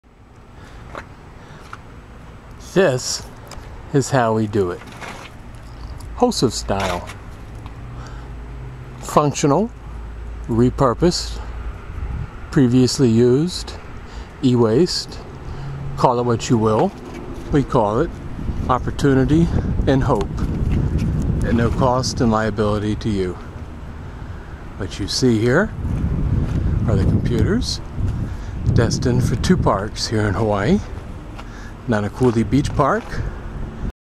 HOSEF Style
This is how we do it. No excuses. No looking for reasons why not. This is a quick overview of the components, loaded in a car, and destined to bring broadband and computers to a Hawaii community park. If I can do, then you can do it.